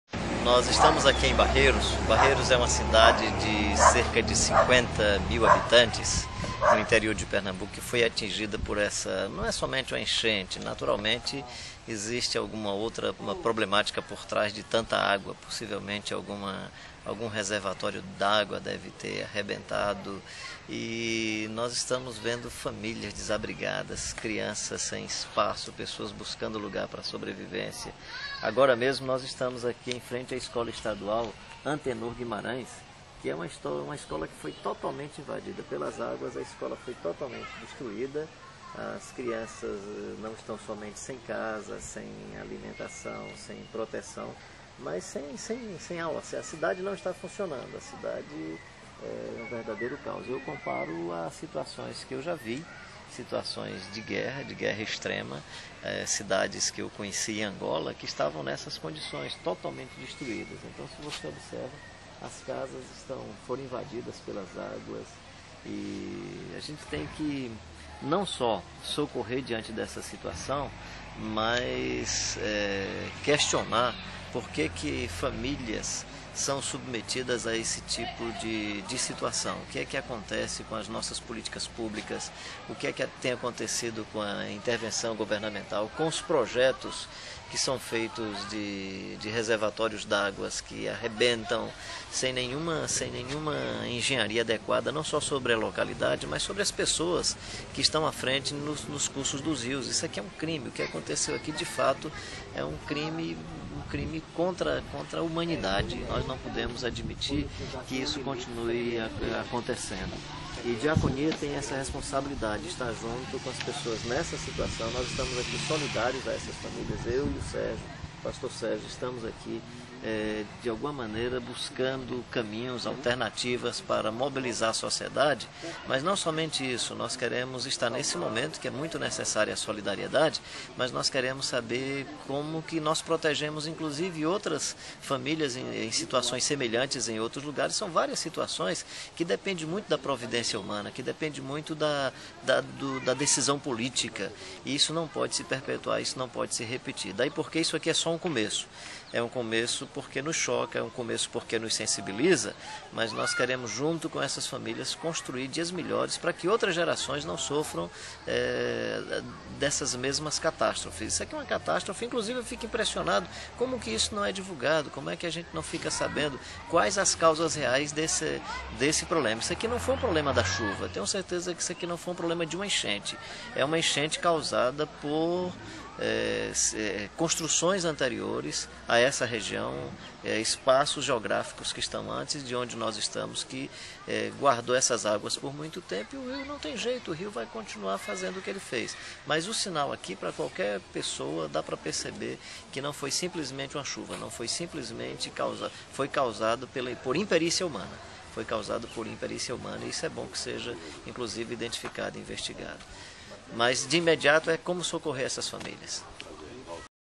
Diretor executivo da Diaconia fala sobre as enchentes em Barreiros.wmv
Pr. Carlos Queiroz comenta o cenário de destruição deixado pelas enchentes em Barreiros, Mata-Sul de Pernambuco.